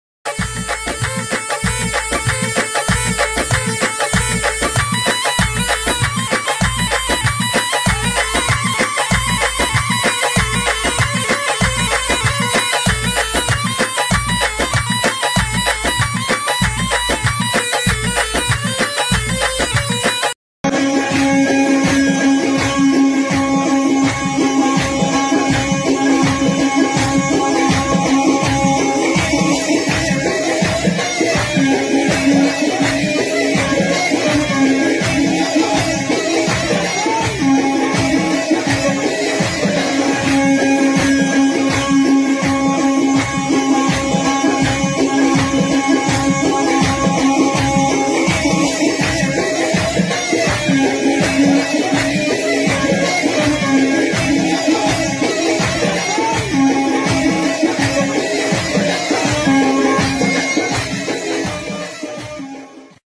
"الكشك" فن ليبي أصيل
هو من الفنون الشعبية في التراث الليبي، تشتهر به قبائل المنطقة الشرقية من البلاد، ويكون حاضرا باستمرار في الأعراس وفي مناسبات الأفراح الأخرى، تعرف أكثر على "الكشك" الليبي.